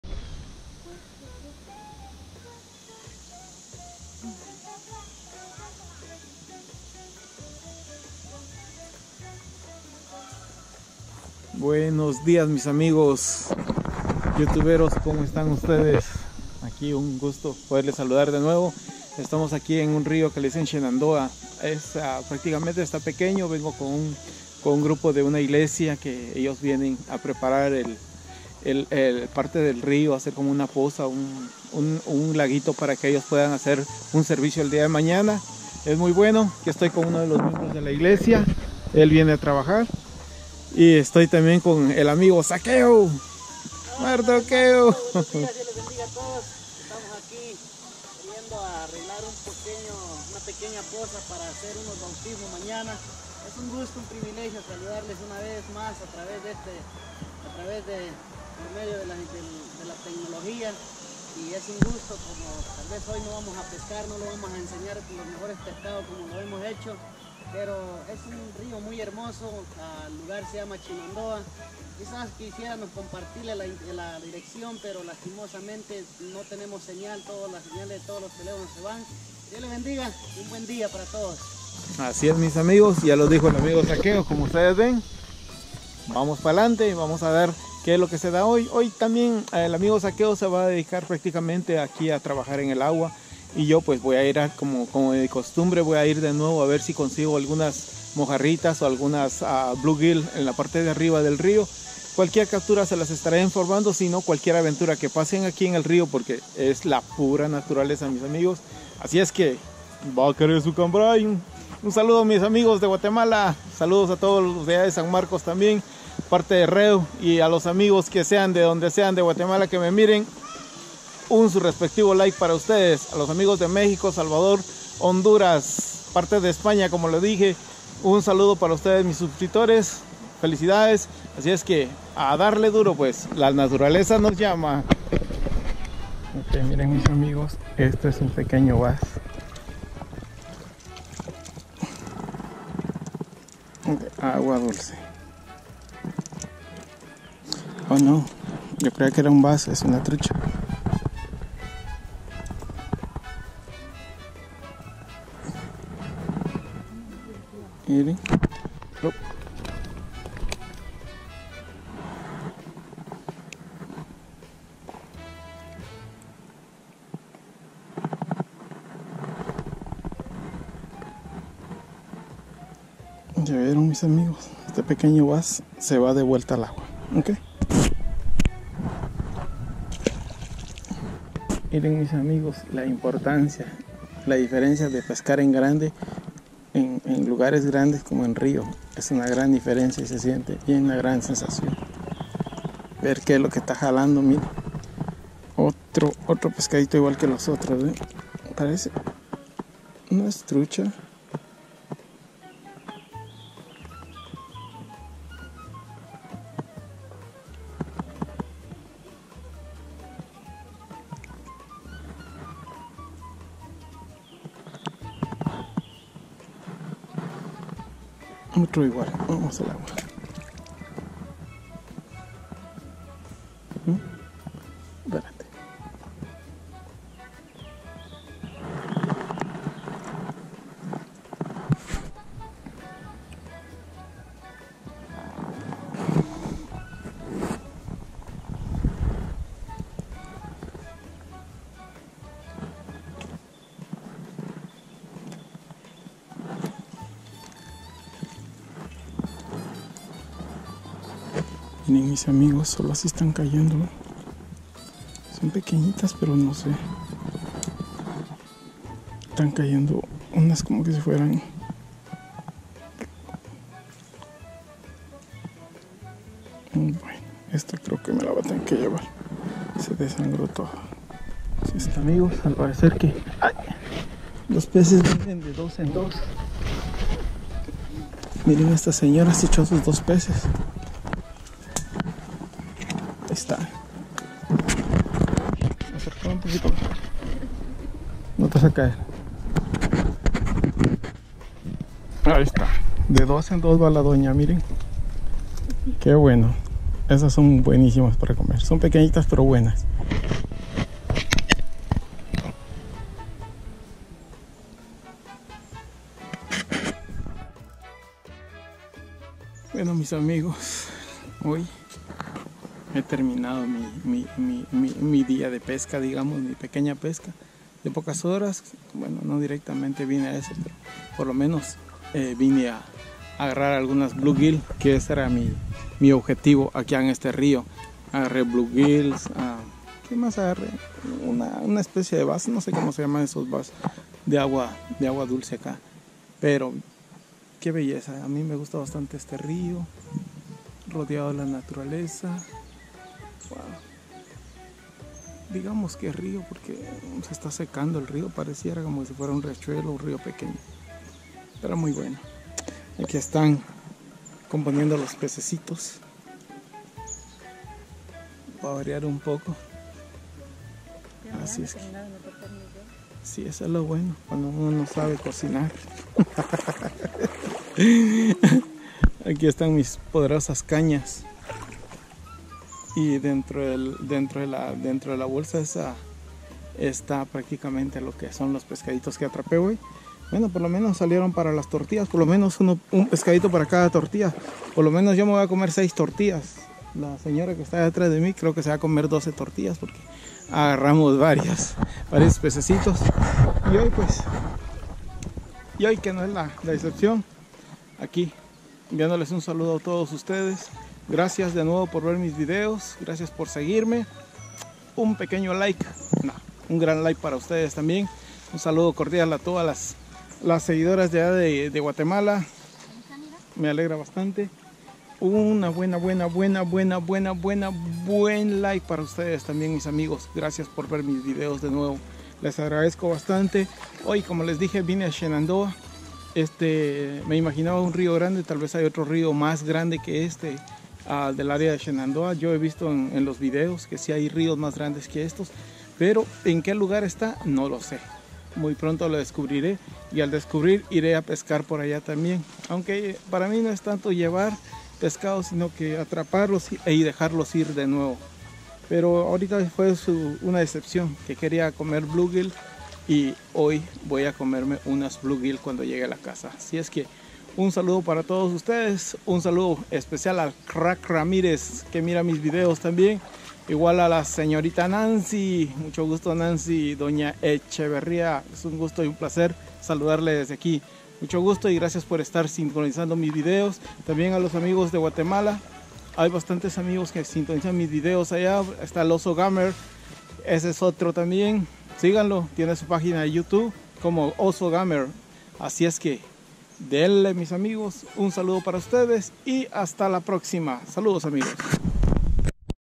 HOY TOCA PESCA EN SHENANDOAH VA. 👍🏾 UN LUGAR BELLO PARA PASARLA EN FAMILIA!!
Gracias al amigo Saqueo y a los amigos De la Iglesia Pentecostés Aposento Alto que me invitaron. En esta ocasión solo capture Bluegill y small bases.